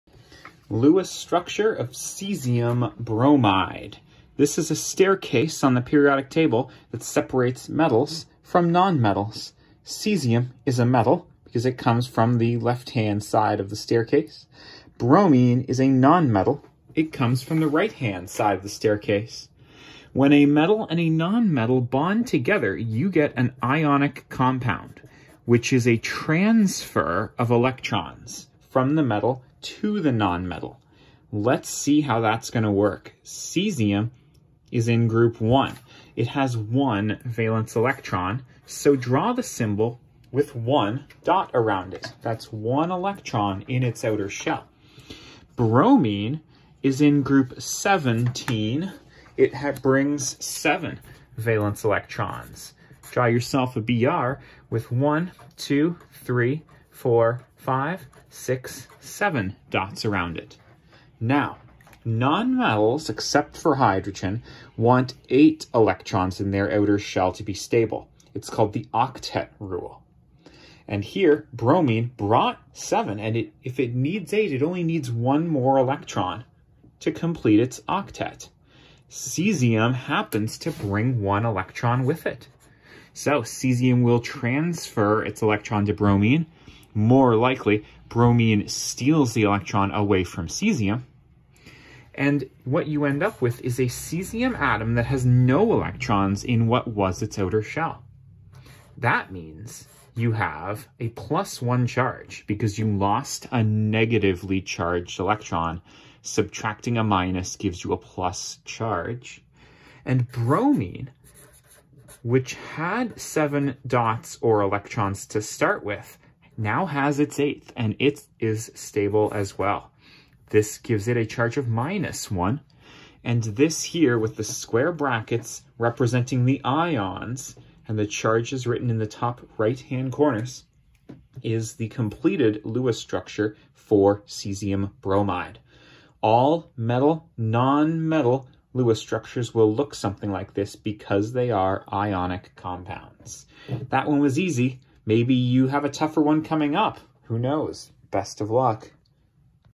Lewis Structure of CsBr, caesium bromide
Cesium bromide is what you get when you mix metallic cesium (Cs) with liquid elemental bromine (Br2).
Cesium is a metal with ONE electron in its outer shell. It wants to lose this electron to become stable.
Bromine is a non-metal with SEVEN electrons in its outer shell. It wants to gain one electron to have a full octet (eight electrons) in its outer shell.
So, only one cesium atom is needed to donate its electron to bromine, and both atoms become stable.
The result is CsBr, cesium bromide, and in this video I draw the Lewis Structure of this ionic compound.